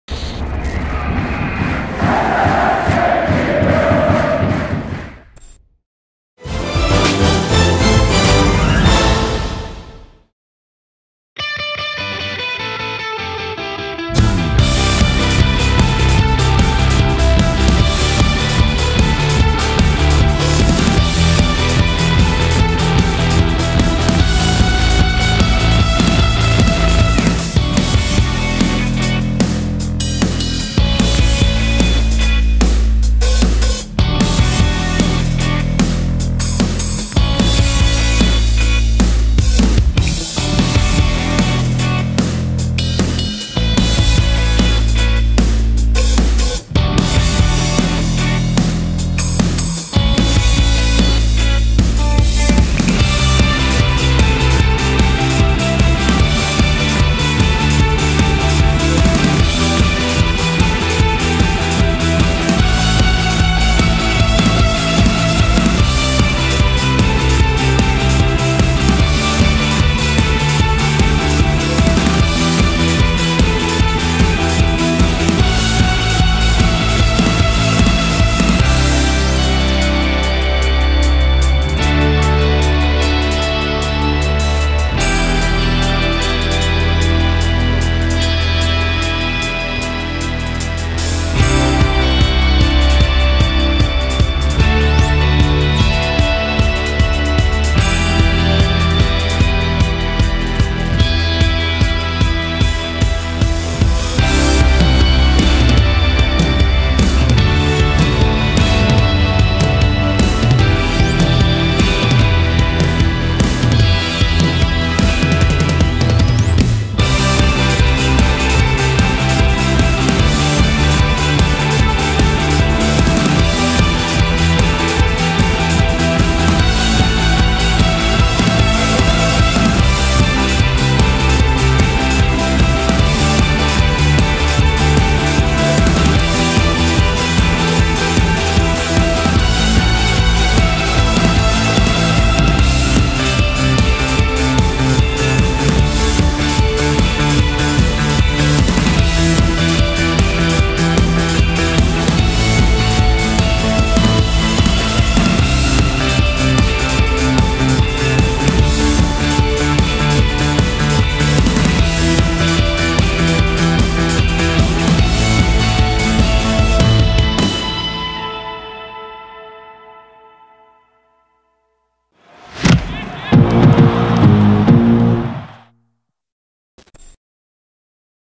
FCI.TV: Die Highlights des Testspiels gegen Unterhaching
Erfolgreicher Abschluss einer intensiven Trainingswoche: Unsere Schanzer bezwangen den Drittliga-Aufsteiger Unterhaching souverän mit 2:0. Die Tore für das Team von Stefan Leitl, das zur Halbzeit einmal komplett ausgetauscht wurde, erzielten Robert Leipertz (38.) und Thomas Pledl (72.). Kurioserweise mussten die Schanzer die zweite Halbzeit zu zehnt bestreiten, da Tobias Schröck sich verletzte und der Unperteiische einen Rückwechsel nicht zuließ.